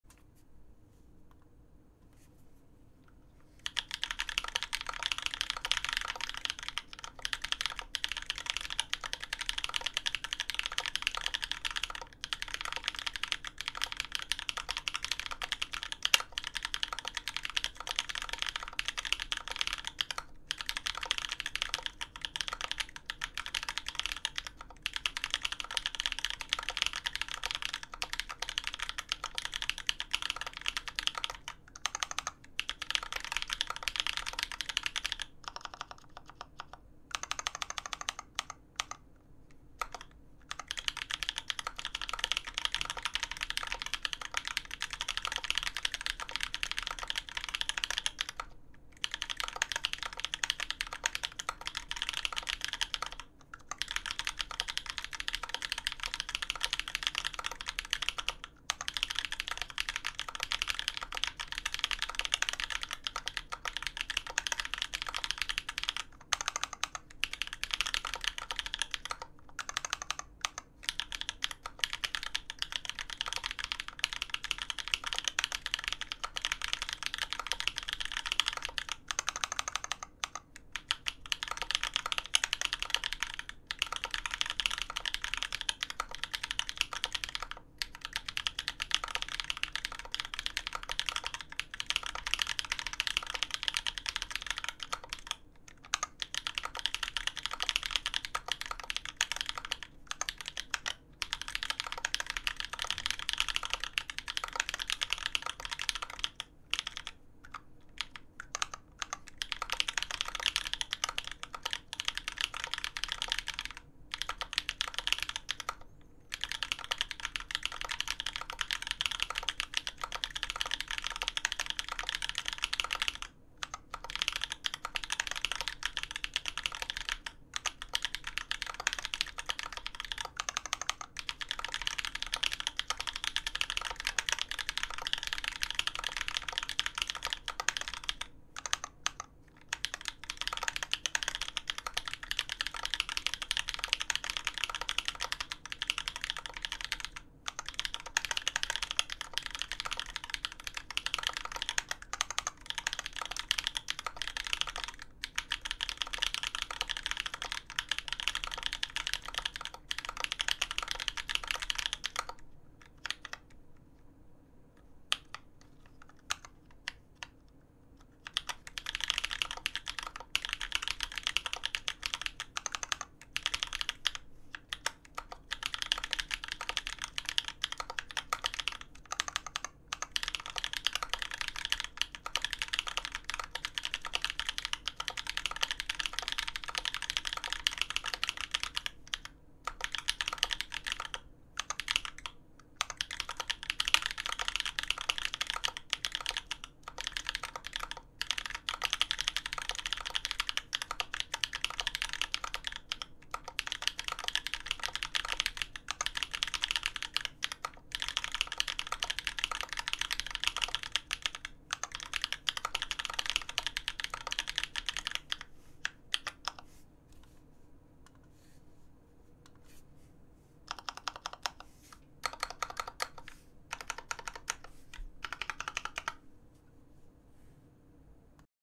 BMEK Alice x HHKB with OG Holy Panda Typing Sounds
⌨️ Keyboard specifications:
- Case: BMEK Alice x HHKB
- Plate: Alu Plate
- Switches: Holy Panda Switch
- Springs: OG Spring
- Lube: Tribosys 3204
- Stabilizers: OA stabilizer (lubed with grease)
- Keycaps: GMK Space Cadet R1

📷 Camera: Sony A7 R3 + 2470GM